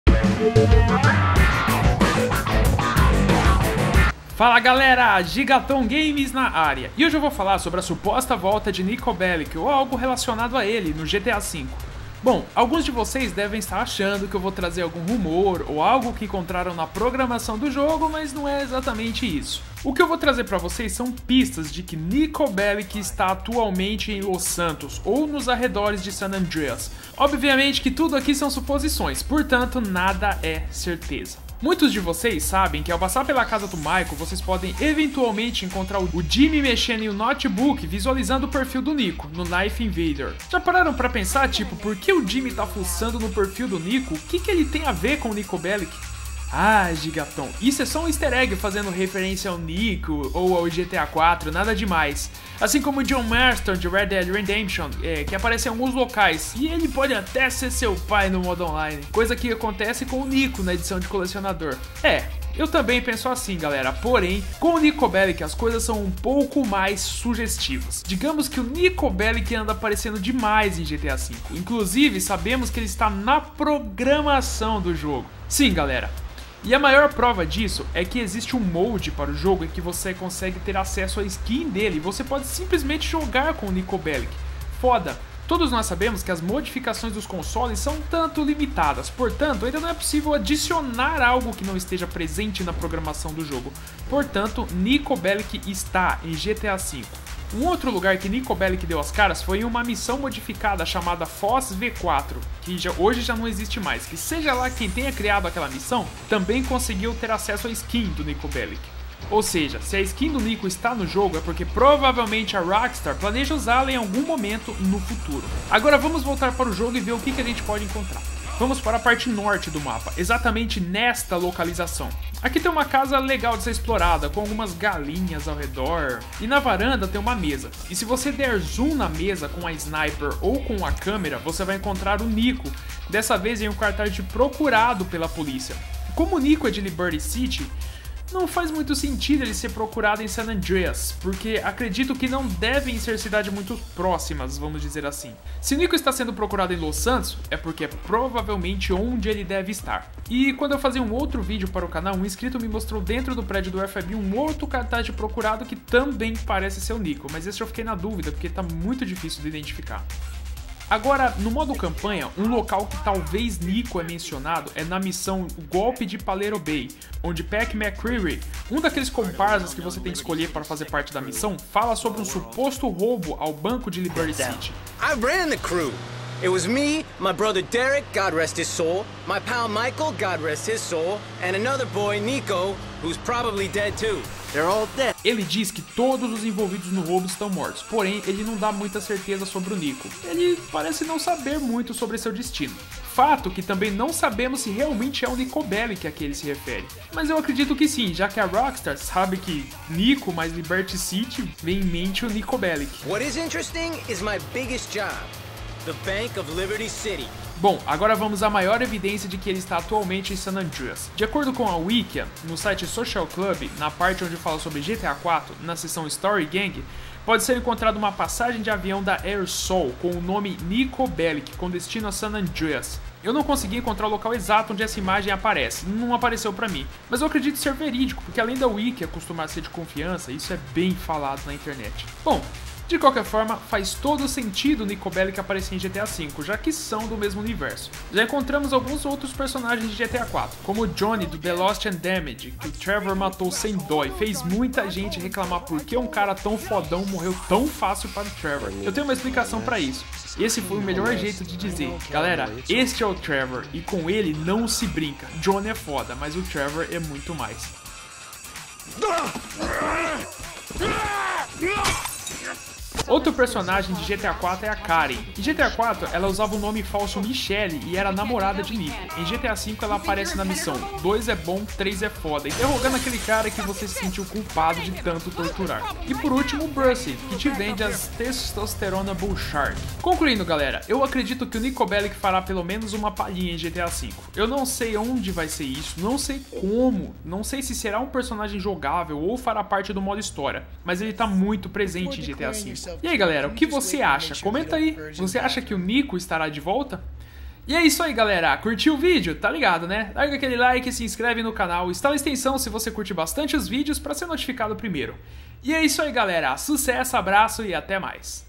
GTA V - NIKO BELLIC ESTARÁ DE VOLTA?
Analisando os indícios da suposta volta do Niko Bellic.